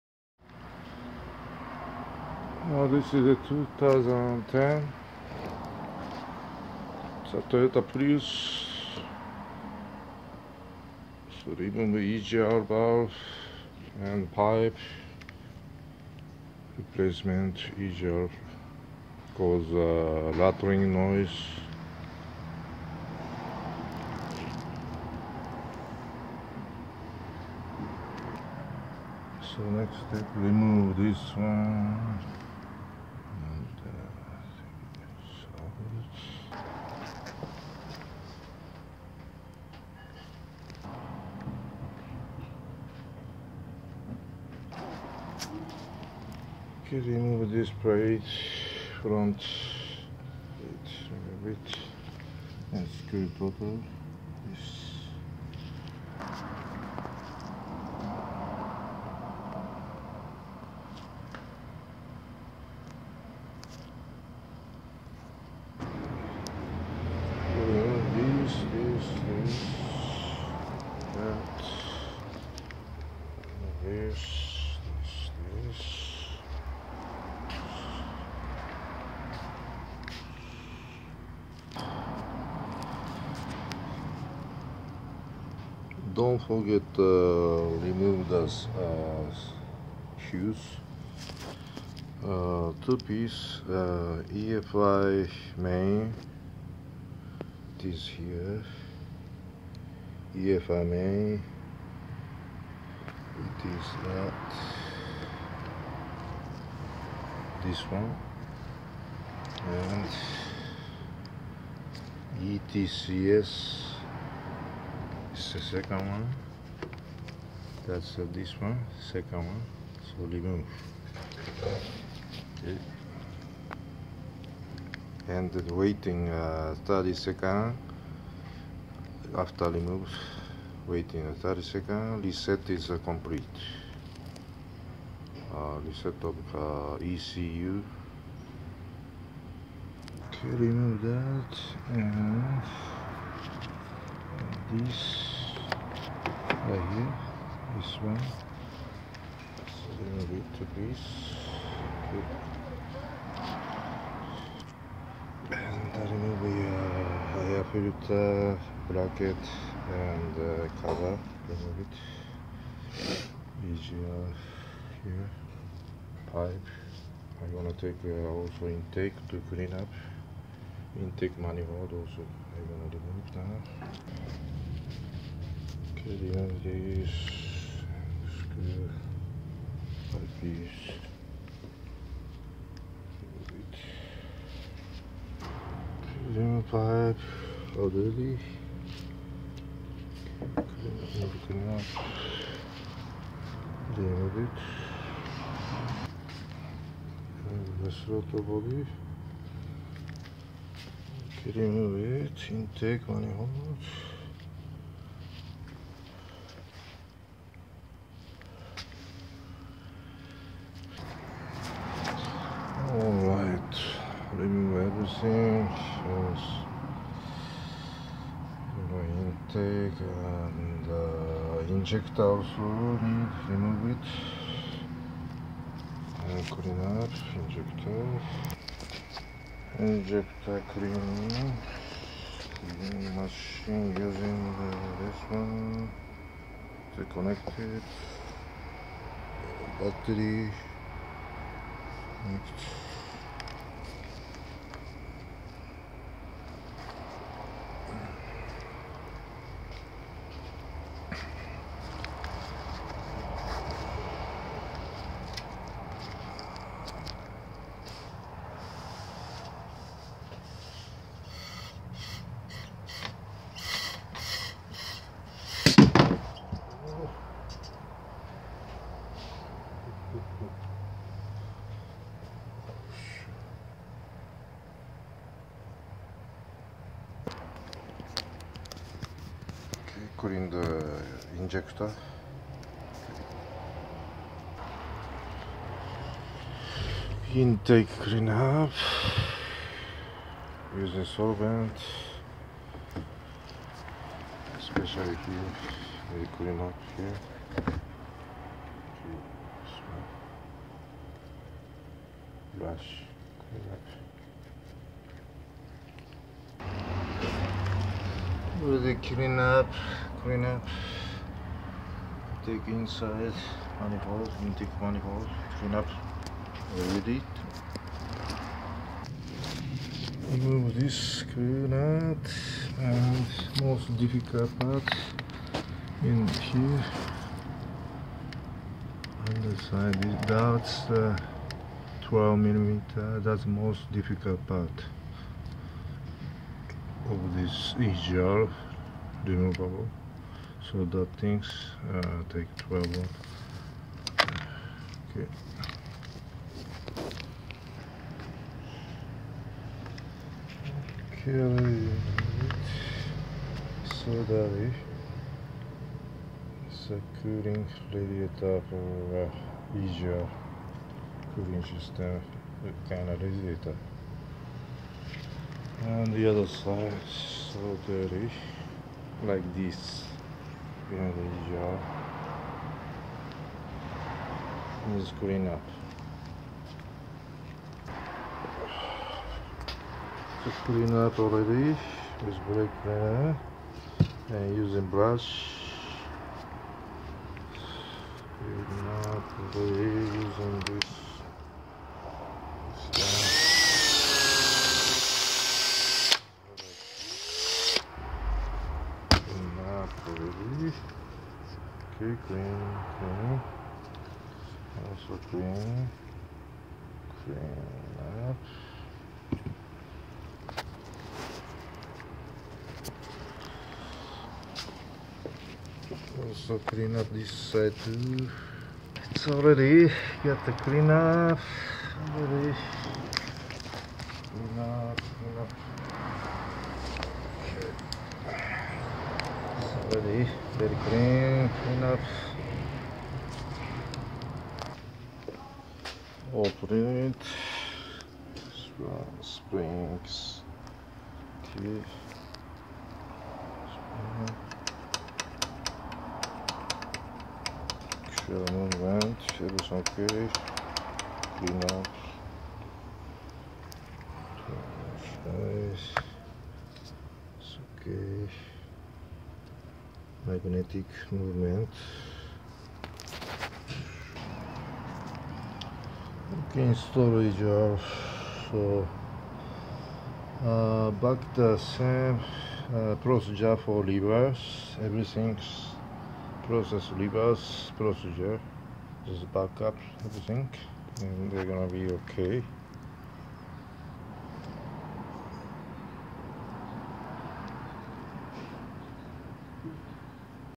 10~15 Toyota Prius EGR problem / プリウスのガタガタ異音の原因と対策！
10~15 Toyota Prius EGR problem / プリウスのエンジンのガタガタ異音の原因と対策！（EGRバルブ清掃方法と異音の修理）/ EGR valve, EGR pipe, EGR cooler, Injector & Intake manihold replacemnet & clean up

The car from 2010 Prius
After repair done no more rattling noise !
Also back to became to normal best condition !